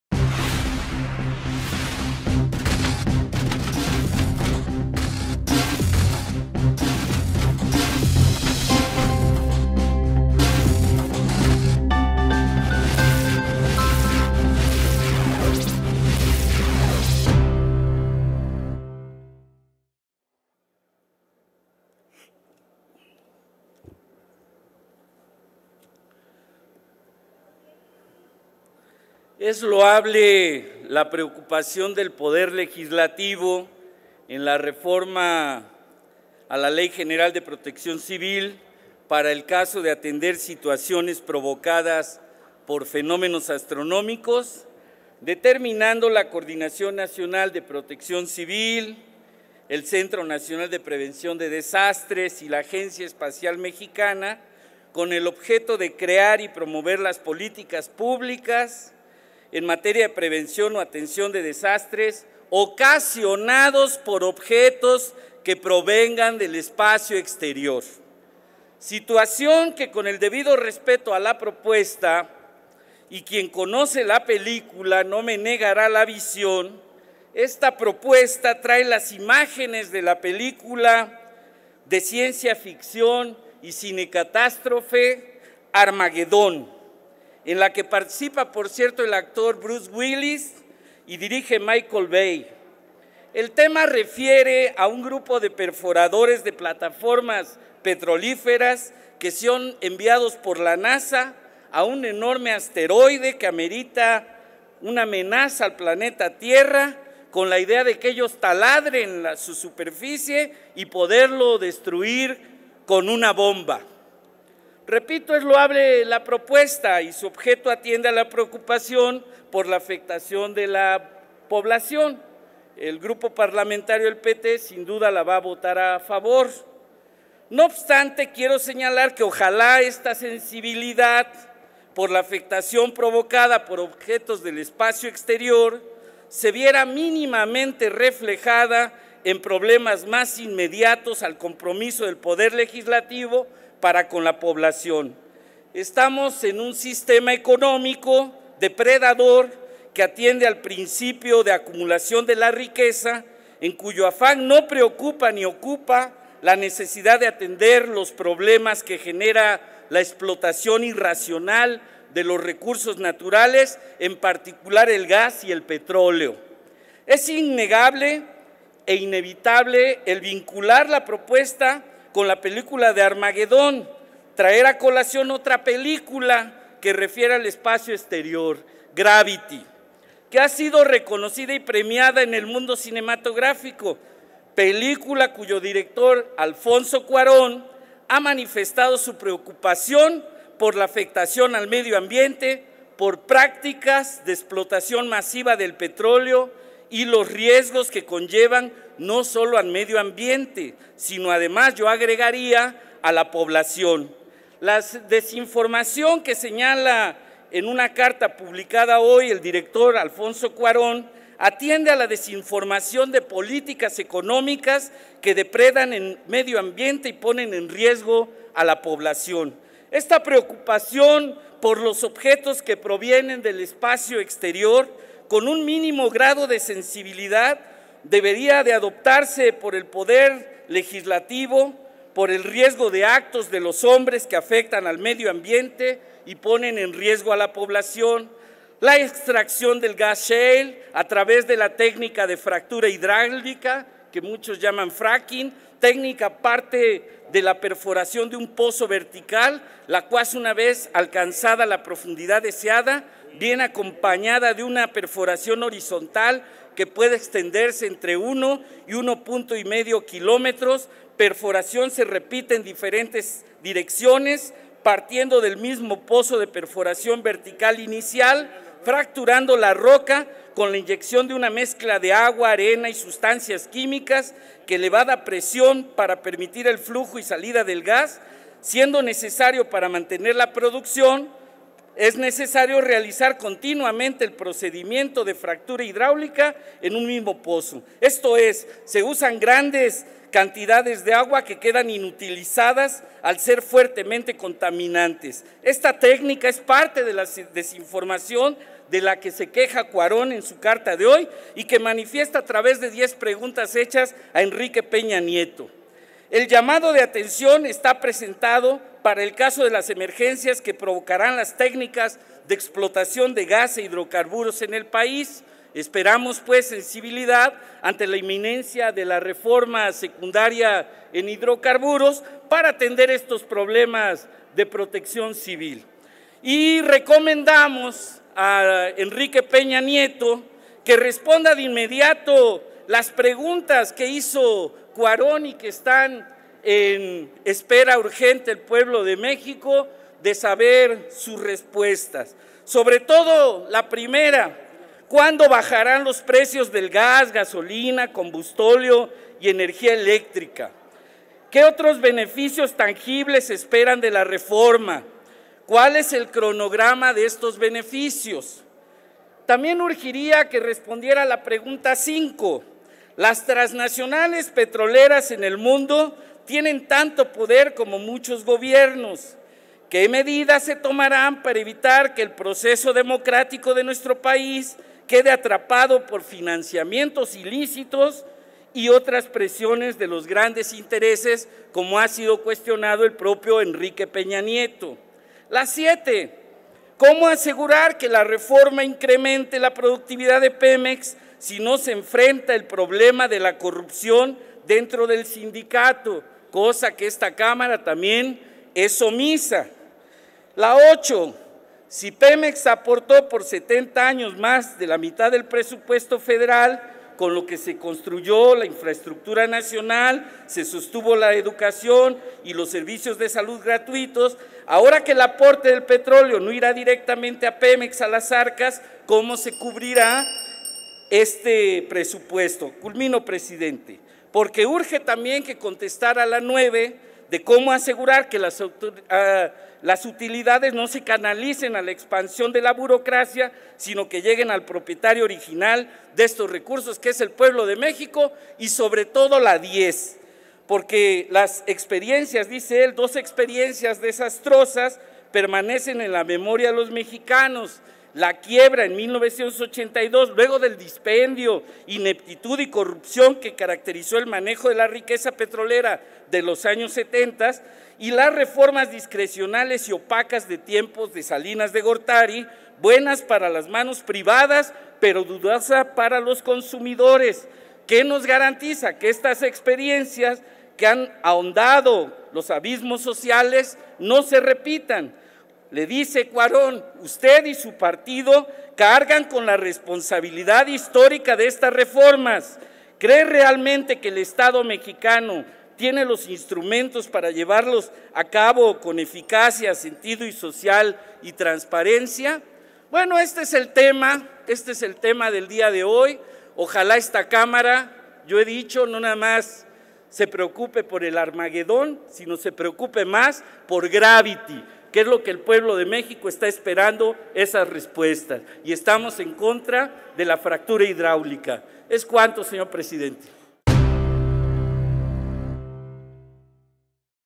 Dip. Manuel Huerta (PT) - Ley General de Protección Civil
El Dip. Manuel Rafael Huerta Ladrón de Guevara (PT) refiere la preocupación del Poder Legislativo en materia de protección civil para la prevención de desastres ocasionados por fenómenos astrológicos. Refiere el desplegado del cineasta Alfonso Cuaron sobre la reforma energética y cuestiona las técnicas de exploración como el fracking. Argumenta sobre las fuentes contaminantes de las técnicas en comento y solicita sensibilidad en la materia durante la expedición de las leyes secundarias respectivas. Finalmente, solicita al gobierno federal dar respuesta a las preguntas realizadas por el cineasta.